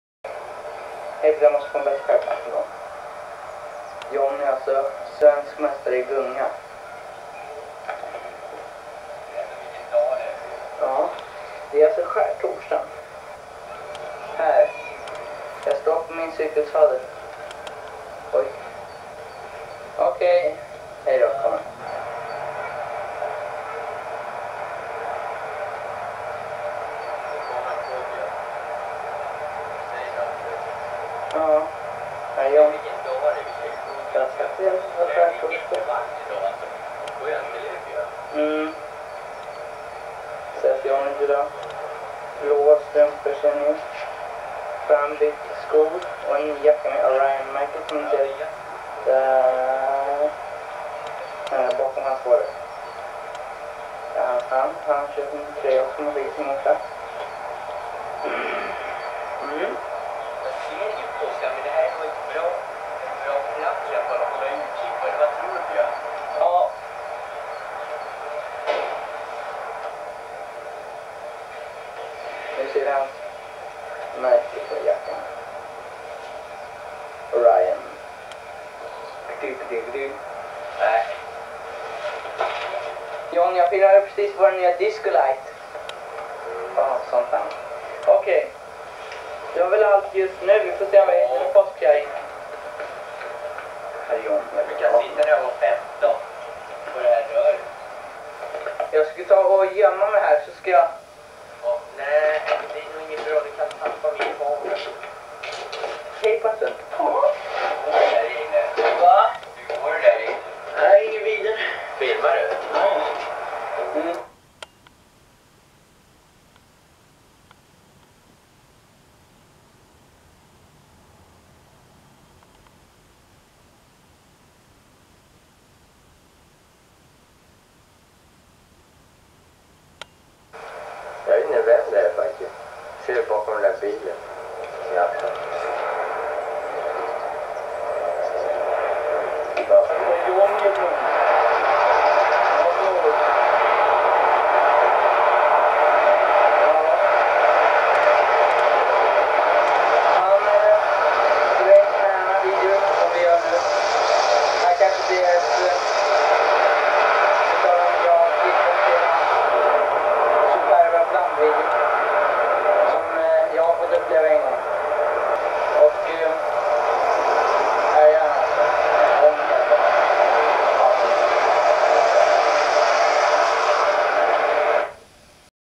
Min 18:e skärtorsdag och april
Skärtorsdag och den första riktigt varma dagen det året. Från mina tidigaste hemvideor, filmade med min gamla videokamera. Mestadels filmat i Stora parken, som den såg ut då. An old home video from when I used to film anything and everything with my old camcorder. I was eighteen here and this was the first really warm weather day that year. It was easter and this was shot on Maundy Thursday.